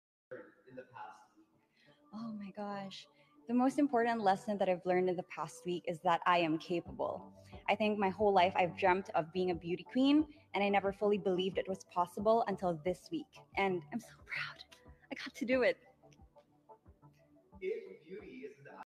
Miss Universe Philippines 2021-San Juan City Rousanne Marie Bernos Preliminary Interview
📽 Credit: @teamfilipinas_